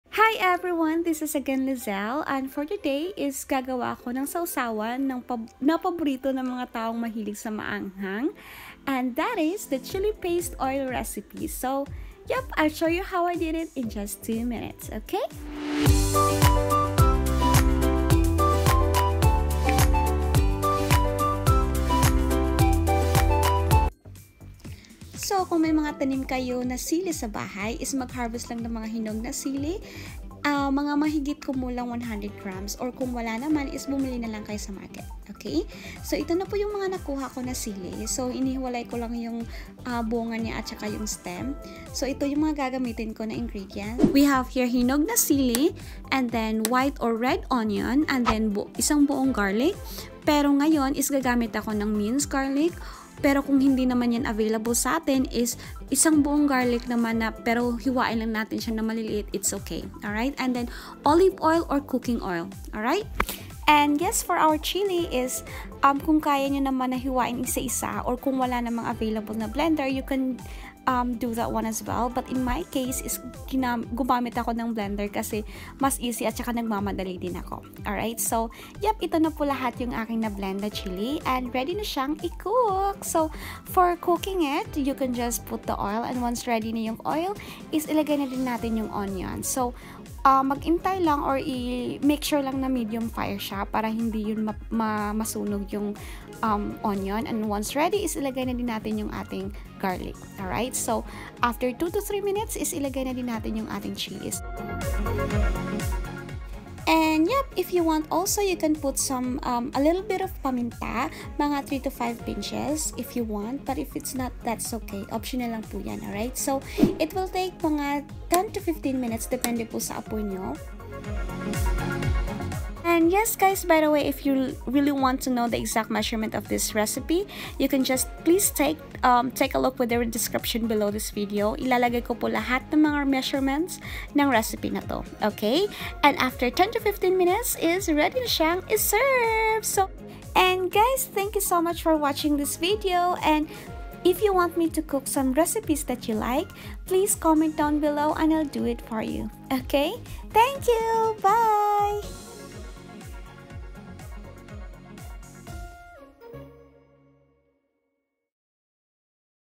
Spicy Bliss in Every Bite! Homemade Chili Oil Recipe
🔥 Turn up the heat in your kitchen with our homemade chili oil recipe! 🔥

Indulge your taste buds in a fiery symphony of flavors as we show you how to create the perfect chili oil from scratch. Whether you're a spice enthusiast or just looking to add some zest to your dishes, this chili oil is a game-changer.

In this video, we'll take you through a step-by-step guide, revealing the secrets to making the most aromatic and mouthwatering chili oil. From selecting the finest chili peppers to mastering the ideal oil infusion, we've got you covered.

This is my easy and basic version intended for beginners. Follow the step-by-step procedure to achieve the same result.

Here are the ingredients for this simple recipe:
100g red chili
1 cup olive oil or cooking oil
1 big size onion
1 big size garlic, minced

Optional:
3 pinches of pepper powder